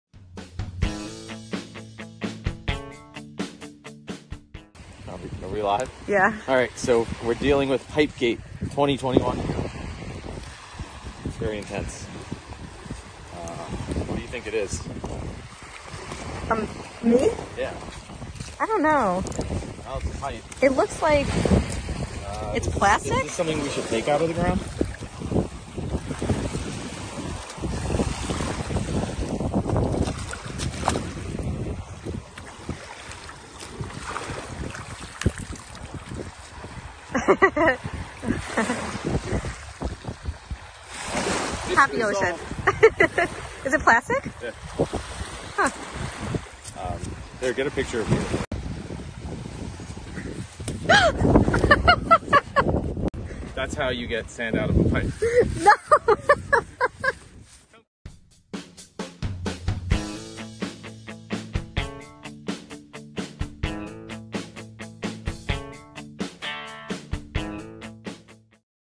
How to Beach Clean Plastic Pipes
Just a fun little short video today about how to clean plastic pipes you might find while beach cleaning. Will be back this week with full trash adventure videos!